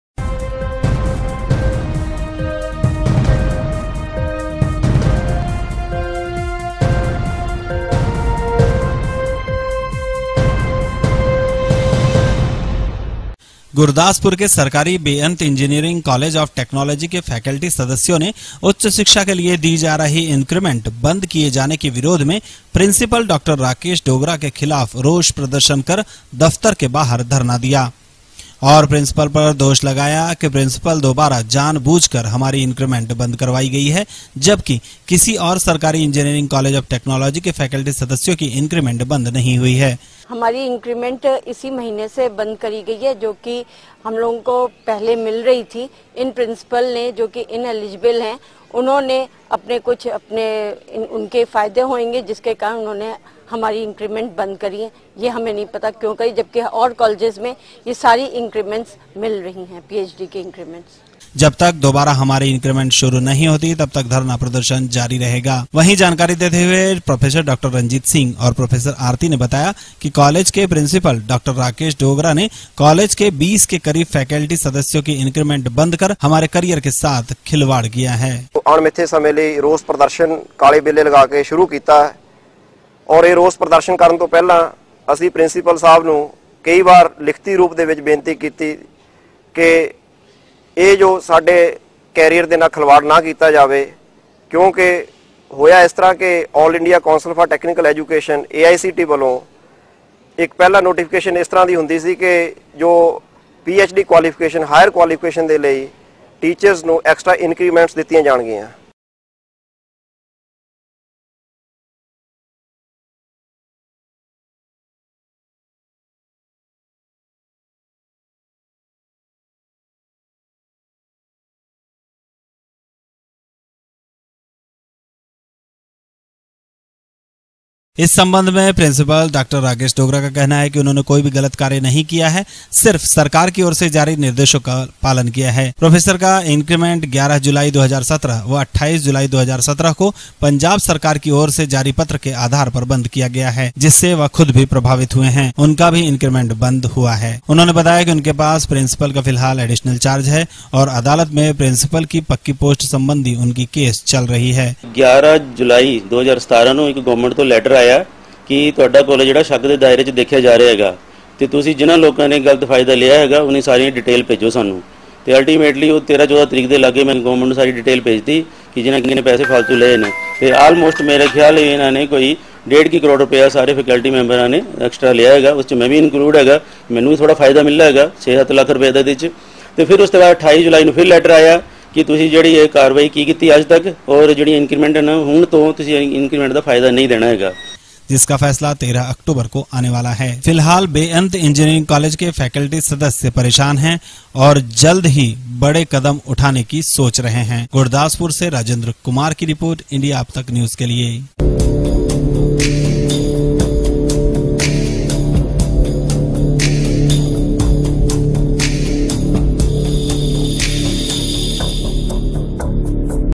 बेअंत इंजीनियरिंग कॉलेज के फैकेल्टी परेशान
गुरदासपुर के सरकारी बेअंत इंजीनियरग कॉलेज आफ टेक्नोलॉजी के फैकल्टी सदस्यों ने उच्च शिक्षा के लिए दी जा रही इंक्रीमेंट बंद किए जाने के विरोध
में प्रिंसिपल डॉ. राकेश डोगरा के खिलाफ रोष प्रदर्शन कर दफ़्तर के बाहर धरना दिया
और प्रसिपल पर दोष लगाया की प्रसिपल दोवारा जान बूझ कर हमारी
इंक्रीमेंट बंद करवाई गई है जब कि और किसी सरकारी इंजीनियरग कॉलेज आफ टेक्नोलॉजी के फैकल्टी सदस्यों की इंक्रीमेंट बंद नही हुई जब तक दोबारा हमारी
हमारी इंक्रीमेंट नही लगती तब तक धरना प्रदर्शन जारी रहेगा

वही जानकारी देते हुए प्रोफेसर डॉ. रंजीत सिंह और प्रोफेसर आरती ने बताया की कॉलेज के प्रिंसिपल डॉ. राकेश डोगरा ने कॉलेज के 20 के क़रीब फैकल्टी
सदस्यों की इंक्रीमेंट बंद कर हमारे कैरियर के साथ खिलवाड़ किया है.